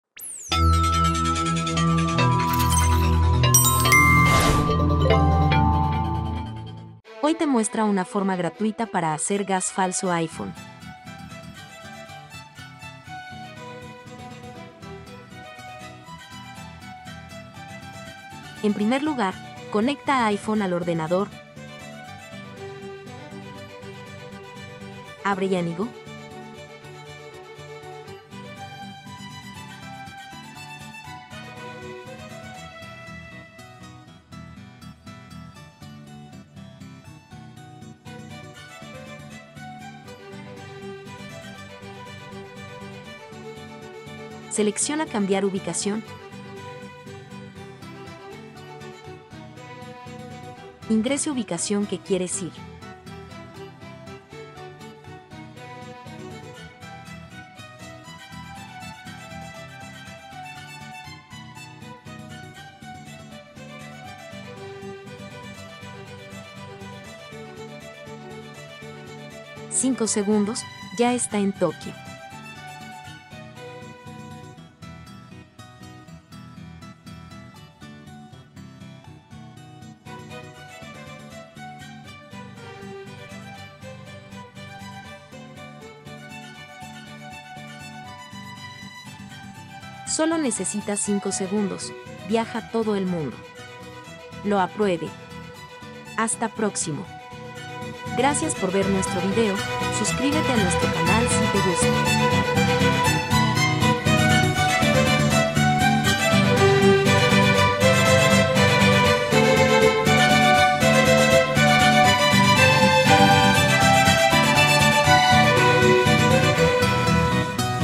🆓CAMBIAR UBICACIÓN 2023｜GPS falso en iPhone gratis PARTE 1🆓
Hoy te muestra una forma gratuita para hacer gas falso iPhone

¡Cambiar la ubicación GPS fácilmente sin jailbreak!

0:00 Introducción
0:50 Paso 1: ingrese nombre de destino